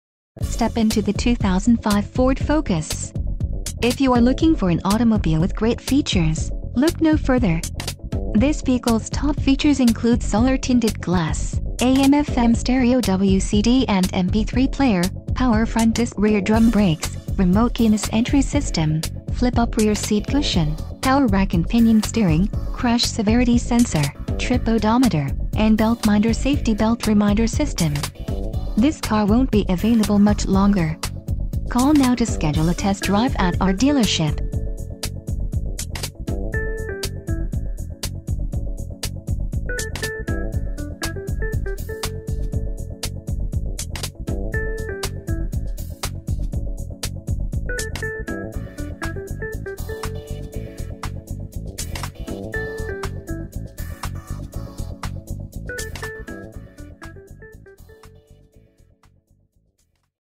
2005 FORD FOCUS - Pahrump Valley Auto Plaza - Pahrump, NV 8
Step in to the 2005 FORD FOCUS. If you are looking for an automobile with great features, look no further. This vehicles top features include Solar-Tinted Glass, AM/FM Stereo w/CD  and  MP3 Player, Power Front Disc/Rear Drum Brakes, Remote Keyless Entry System, Flip-Up Rear Seat Cushion, Power Rack  and  Pinion Steering, Crash Severity Sensor, Trip Odometer, and Belt-Minder Safety Belt Reminder System. This car won't be available much longer.